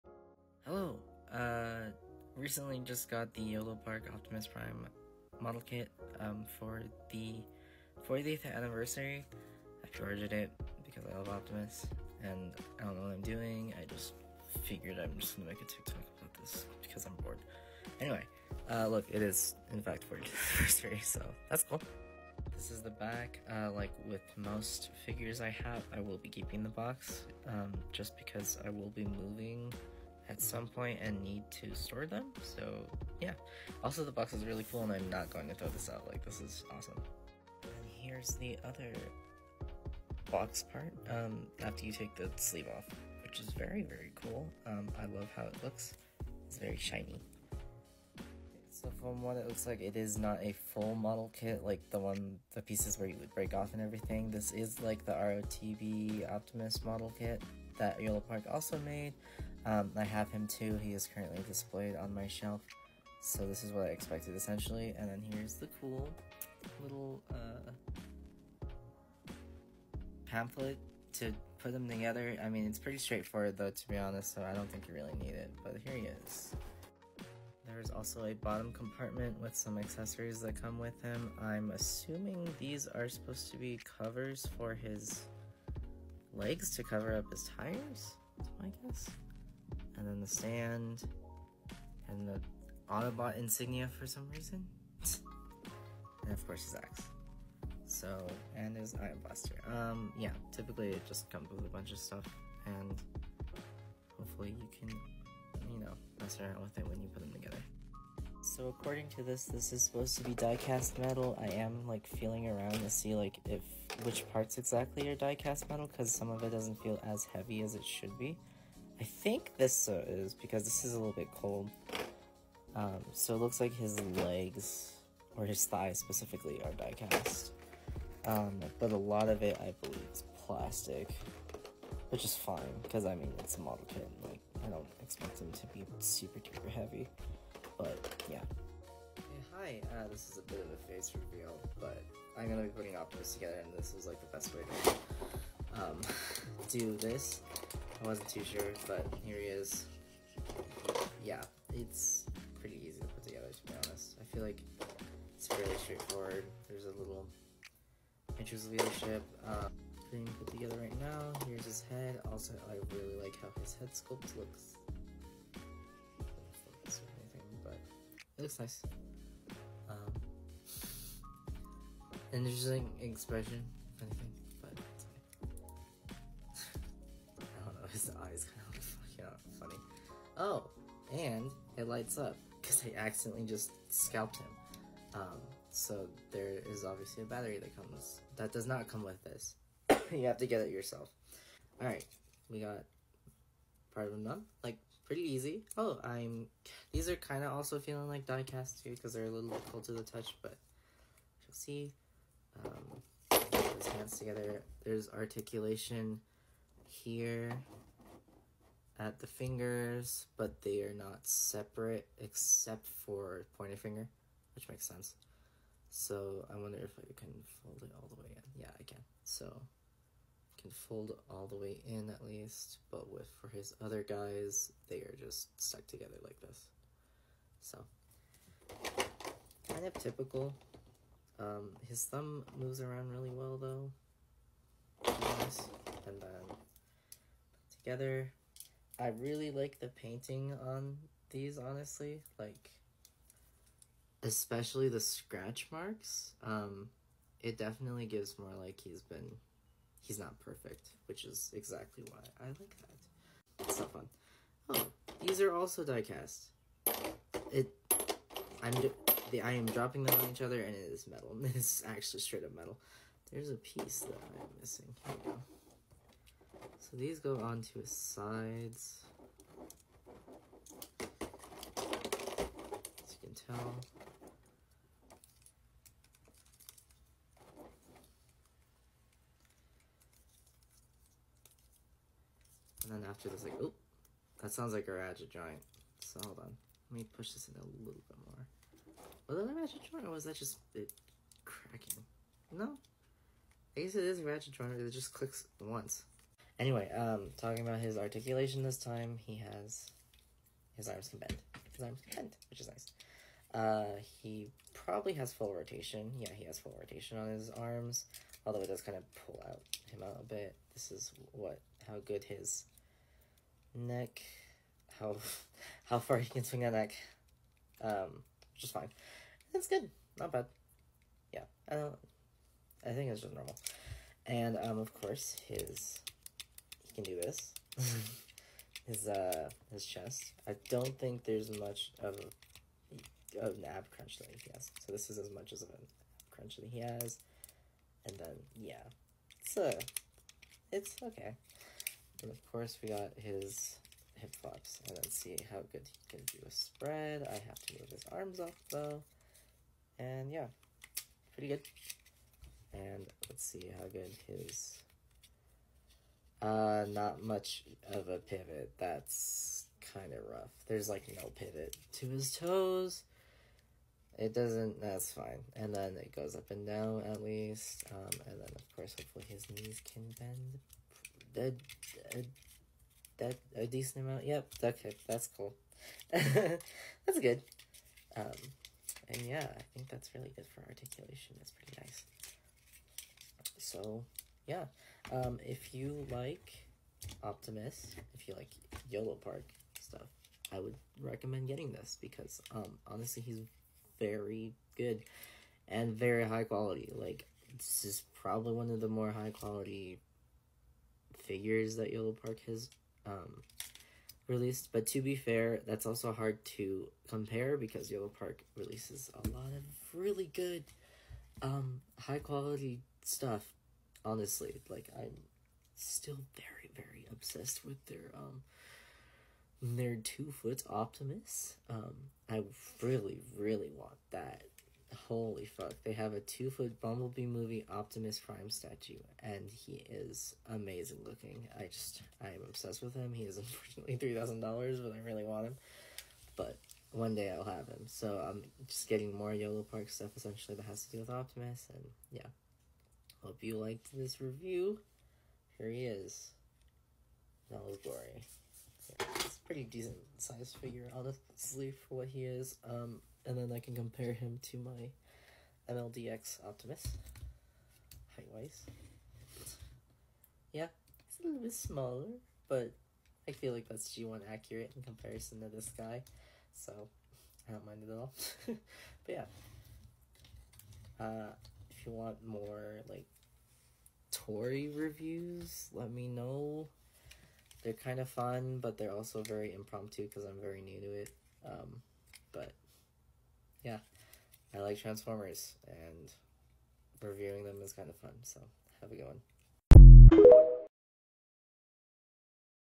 Yolopark Diecast Optimus Prime Model Kit | Kage Reviews
Originally made this for TikTok and wanted to upload it here, I will hopefully be reviewing more TF toys in the future since it seems fun! I also have more Transformers themed content in mind since that's my main interest right now.